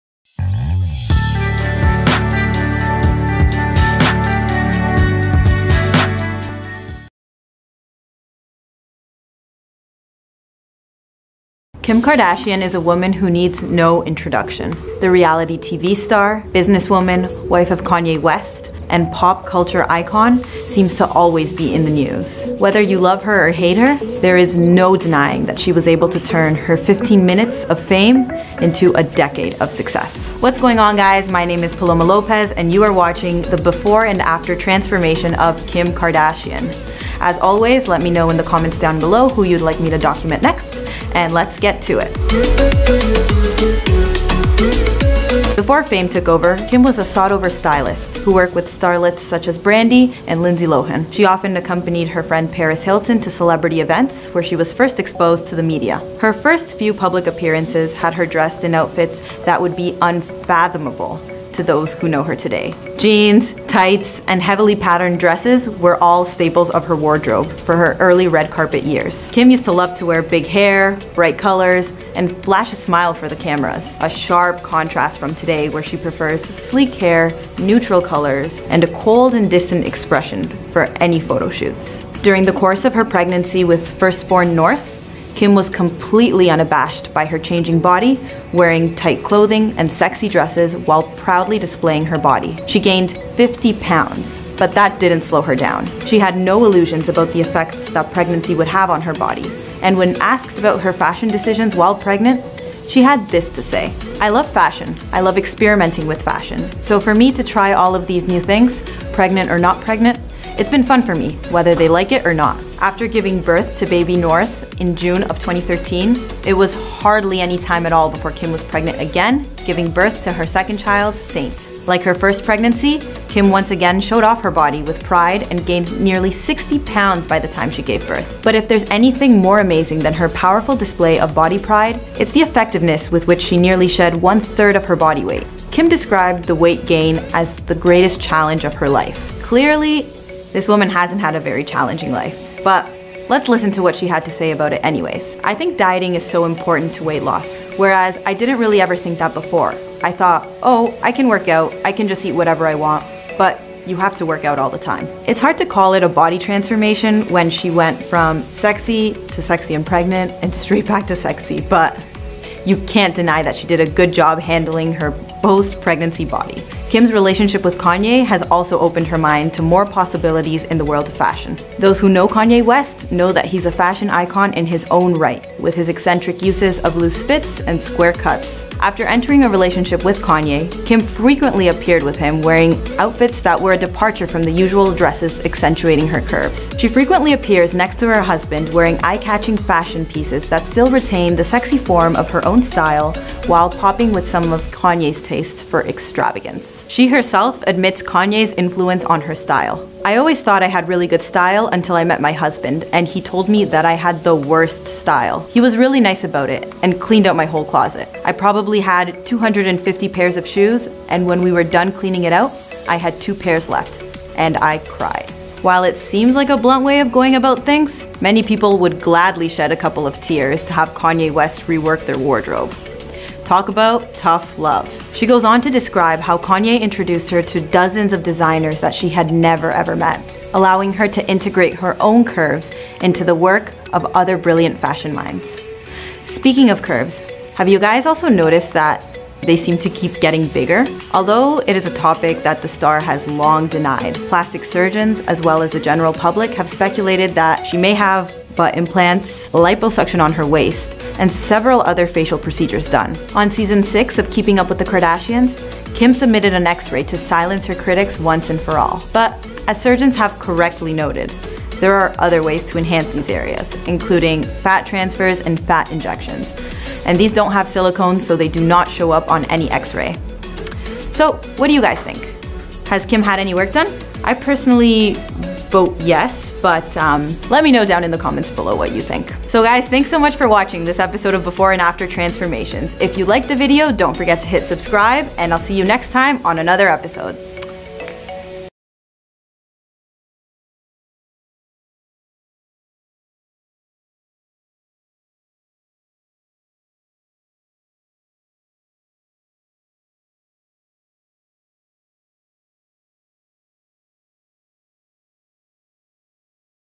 KIM KARDASHIAN | Before and After Transformations
Welcome to : Top Stars

Big thanks to channel: Famous Entertainment For their wonderful content
Arguably the biggest name to come out of the Kardashian empire is Kim Kardashian, wife to Kanye West and mother to North West, Saint West and Chicago West, sister to Kourtney, Khloe, Kylie (Kylie Cosmetics), Kendall and Rob and daughter of Kris Jenner and the late Robert Kardashian, well known for being O.J Simpson's attorney in the trials.

Kim Kardashian recently caught some attention for the  like butter meme following the release of the Yeezy Boost 350 V2 sneakers.

Here's her Before and After Transformation, including possible plastic surgery